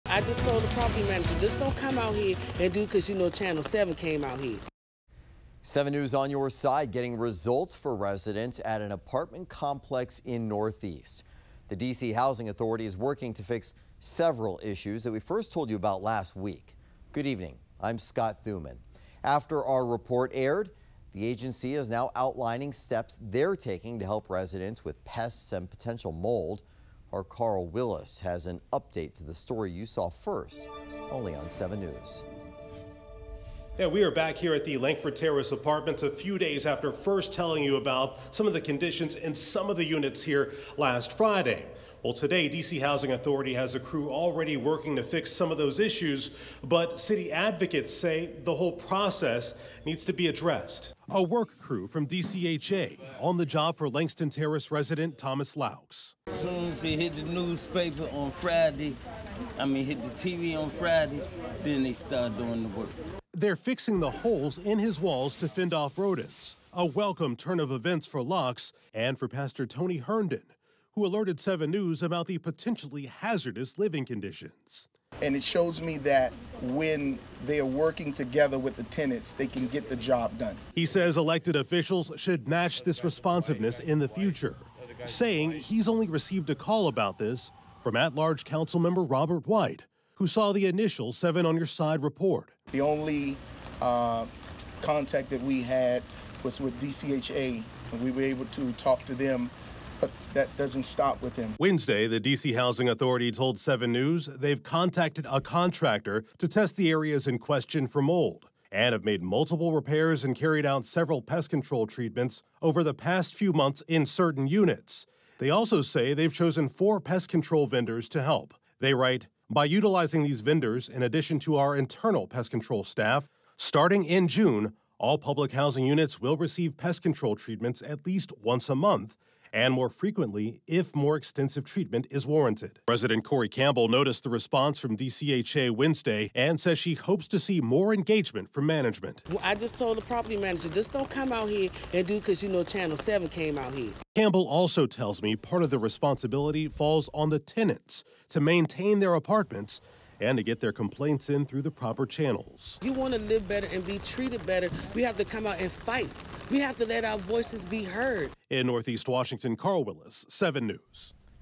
DC crews fix issues at federally-funded housing project after 7News reports on poor conditions
7News On Your Side returned to a Northeast D.C. apartment complex Wednesday after first reporting about the conditions in units at the federally-funded housing project. We spotted a DC Housing Authority crew at the property working to fix some of those issues including holes in the walls that allowed rodents to enter.